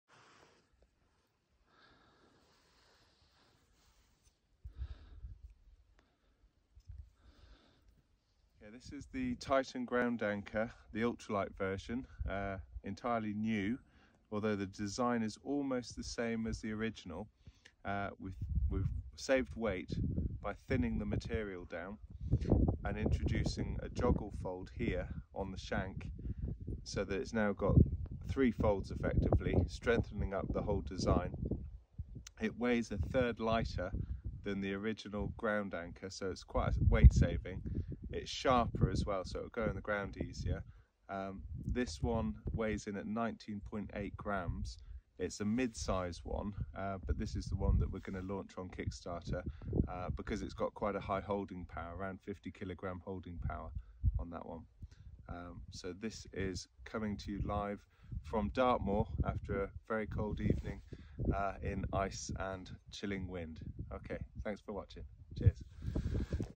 Titan Ground Anchor the Ultralight version 19.8g holds 50kg high performance tent peg
A third lighter than the standard version of the same size (medium) they can hold over 2500 their own weight, shallow ground penetration so misses hitting rock. pre order only for the worlds best tent pegs